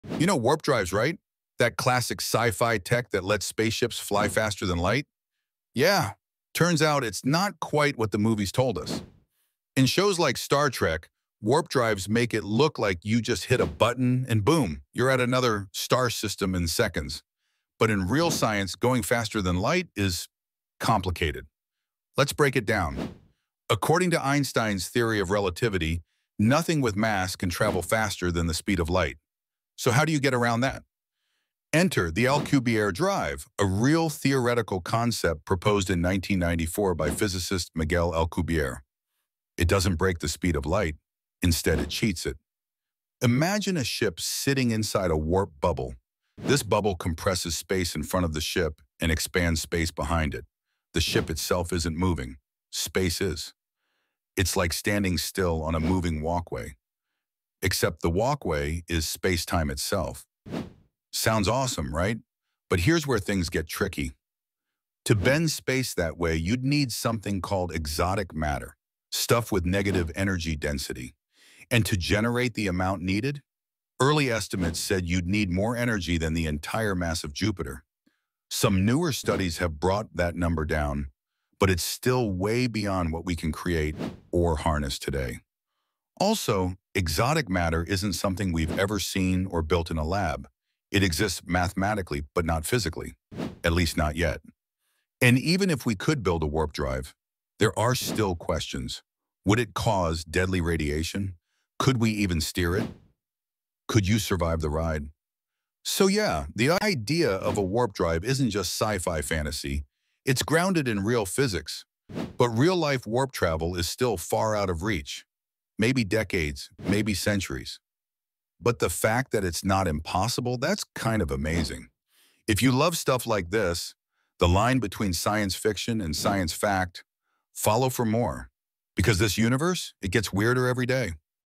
The Truth About Warp Drives (It’s Not What Sci-Fi Told You)
Ever wondered how warp drives could actually work? Discover the fascinating truth behind the sci-fi tech that lets spaceships soar faster than light—turns out, it's not quite like the movies! Unpack the complexities of Einstein's theory of relativity and the theoretical Alcubierre Drive, which proposes a unique way to cheat the speed of light using a ‘warp bubble’ to alter space around a stationary ship. Dive into the challenges of needing exotic matter, a mysterious substance yet to be observed in reality, and the staggering energy requirements that surpass anything we can harness today. Plus, explore the unanswered questions surrounding potential dangers like radiation and navigation. While real-life warp travel is still a dream for future generations, the prospect that it isn't impossible makes the universe all the more intriguing. If you're curious about where science fiction meets science fact, follow along for more mind-bending insights in a universe that's always getting stranger!